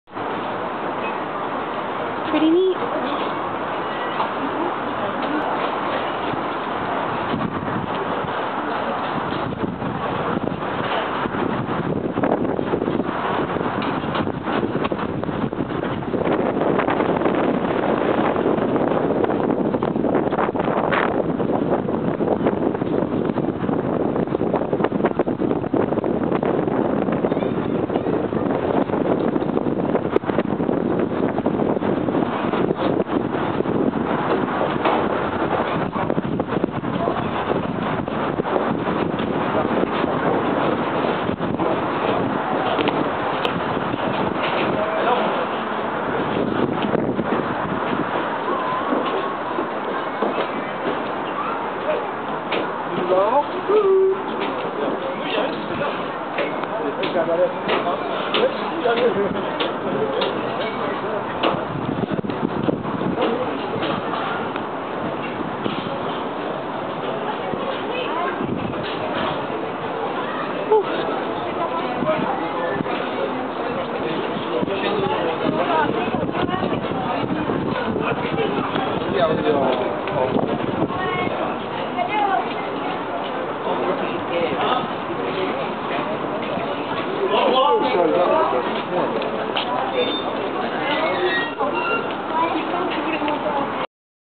Eiffel Tower - view from second level
this video was taken by running around the second level of the Eiffel Tower.  Nov 29, 2009